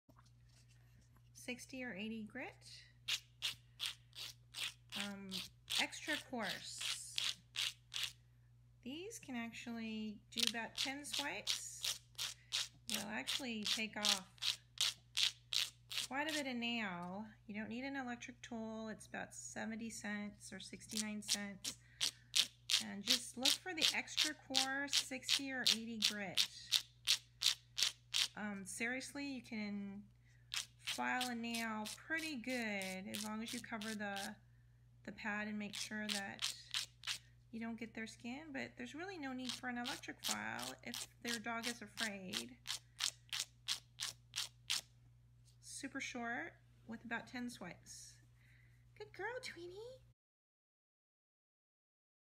LAP GROOMING: Using a Coarse Nail File to Shorten Tweety's Nails - Maltese Nail Filing말티즈미용
LAP GROOMING -- sorry about the quality == it was very late in the evening and we just shot it with my iphone as Tweety was relaxing on my lap **(^-^)**

We don't always feel the need to use the cat scissors or electric nail file.    Most evenings we can do our evening brush outs on our LAP and we can take a few swipes on their nail with an EXTRA COARSE NAIL file and take some good length off with a few to 10 gentle swipes!

We will source our nail filing supplies on our Maltese DOG BLOG

Groomer/Stylist:   MOH aka MOM
Music and Artistic Director:   MOE aka EM
Team Maltese:   Tweety, Cosette, Dolce and Pinky    aka Tweety the Sweety, Cosy Cosette, Dolce da Diva and Pinky the Stinky

Music/Artisitc Director:   Em